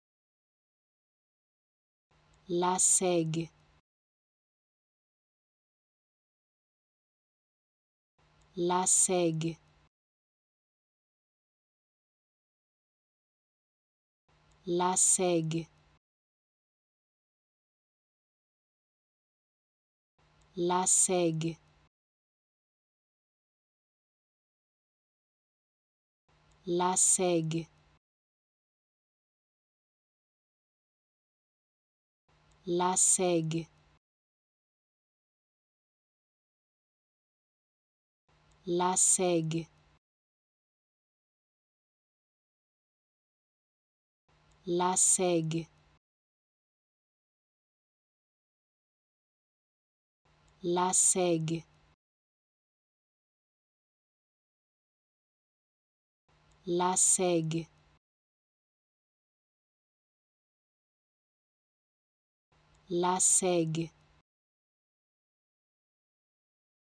How to Pronounce ''Lassègue'' Correctly in French
Learn how to say and properly pronounce ''Lassègue'' in French with this free pronunciation tutorial. Apprenez à prononcer des mots en français grâce à nos tutoriels.